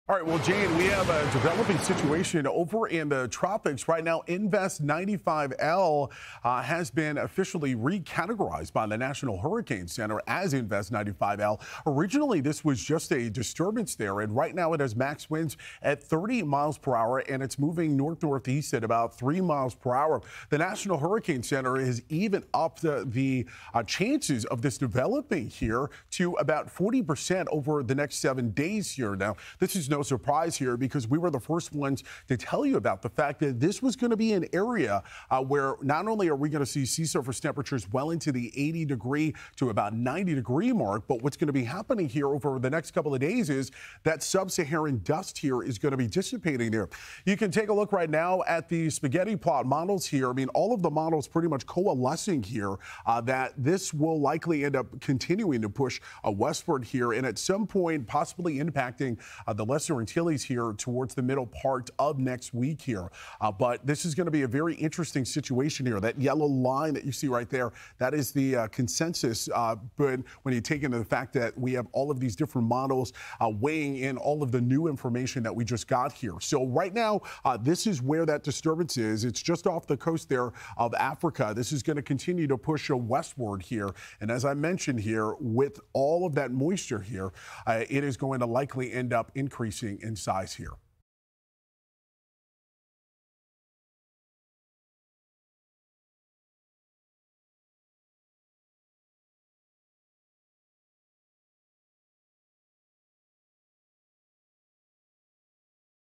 Invest 95L Forms
A cluster of thunderstorms off Cape Verde Islands now has been designated Invest 95L by the National Hurricane Center and given an increased chance of development over the next 7 days.